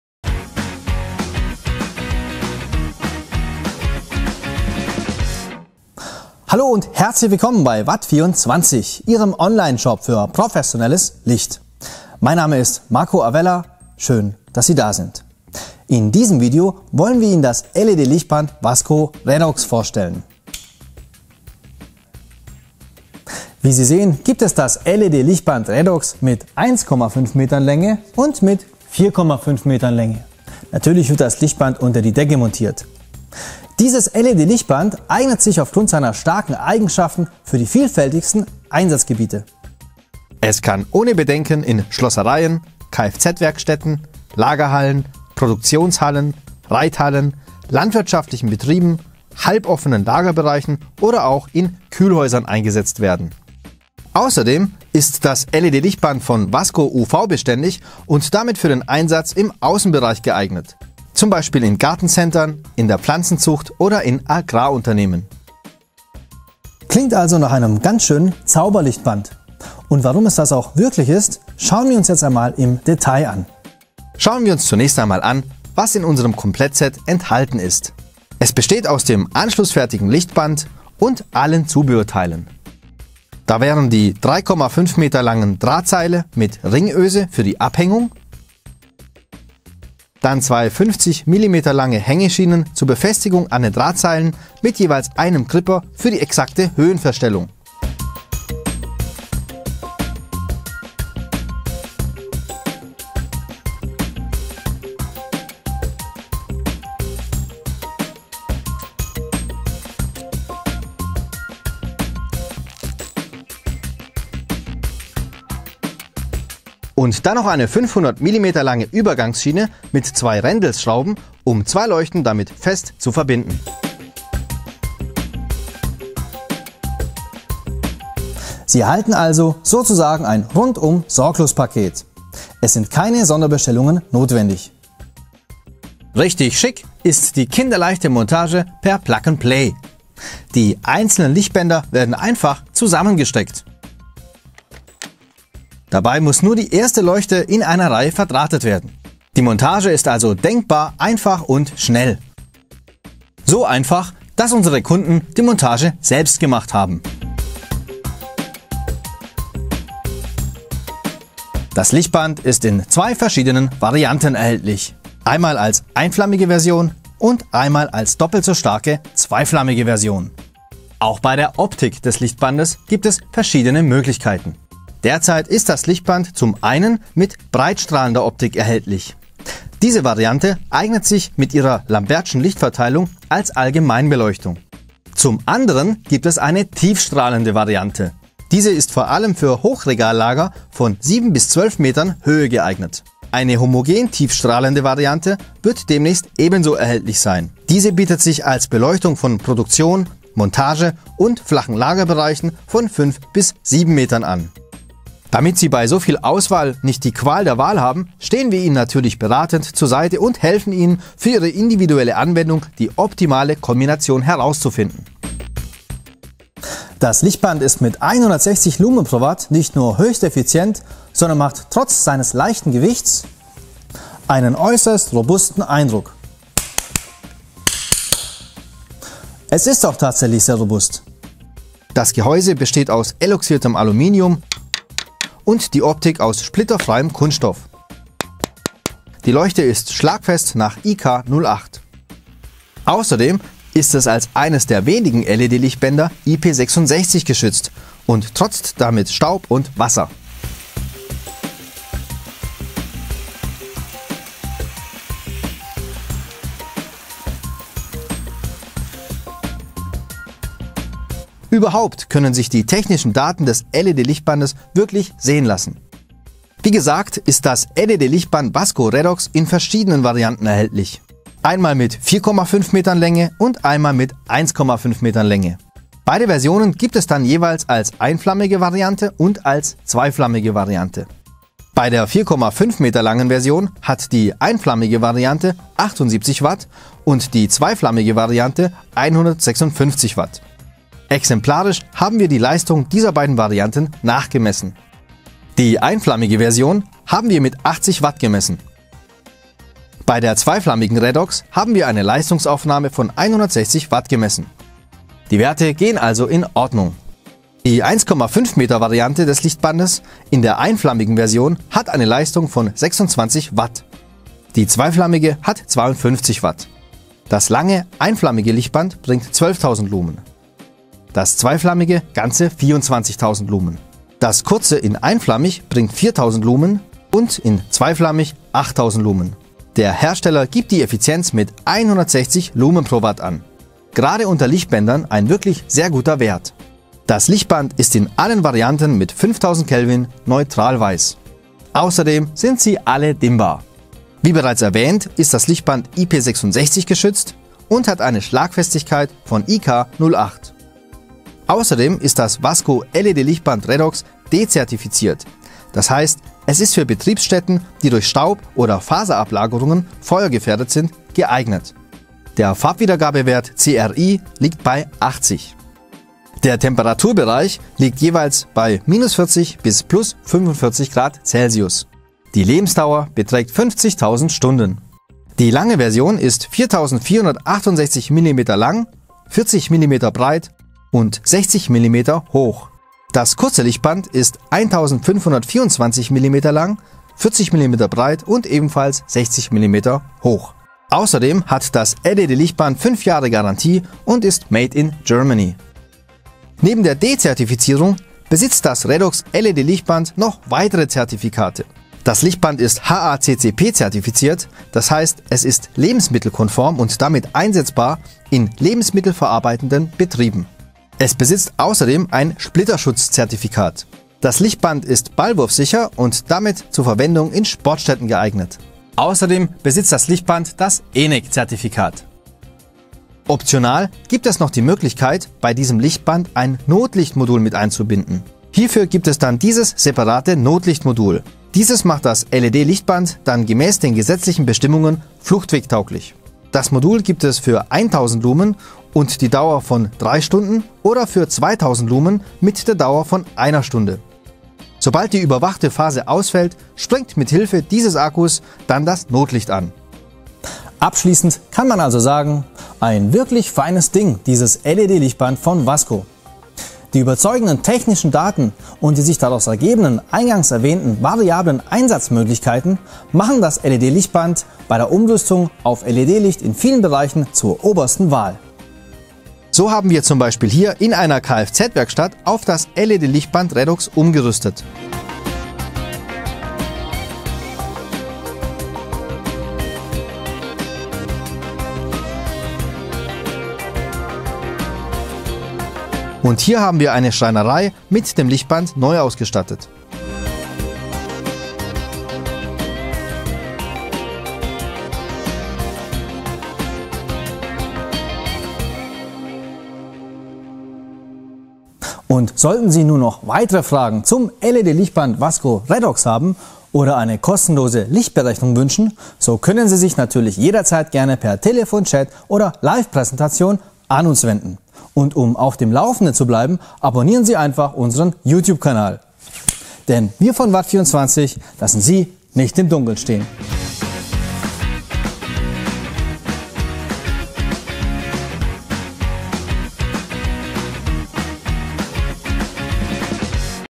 LED-Lichtband Wasco REDOX – Das starke LED-Lichtband für moderne LED-Beleuchtung
Das LED-Lichtband Wasco REDOX kommt anschlussfertig im Komplettset mit allen Zubehörteilen.
In diesem Video zeigen wir Ihnen das LED-Lichtband REDOX und das Zubehör im Detail, erklären die Eigenschaften und zeigen Beispiel-Anwendungen.
Das Lichtband ist IP66 geschützt und hat eine sehr hohe Energieeffizienz von 160 lm/W.
Es ist UV-Beständig, daher für den Außenbereich geeignet und hat das D-Kennzeichen und ist damit für den Einsatz in feuergefährdeten Betriebsstätten geeignet.
Das Lichtband ist in verschiedenen Längen, Lichtstärken und mit unterschiedlichen Optiken erhältlich.
Anwendung findet es als Beleuchtung in Werkstätten, Schreinereien, Lagerhallen, Kühlräumen, landwirtschaftlichen Betrieben, Kfz-Werkstätten,
Es ist ideal für die Umrüstung oder Neuausstattung mit LED-Beleuchtung.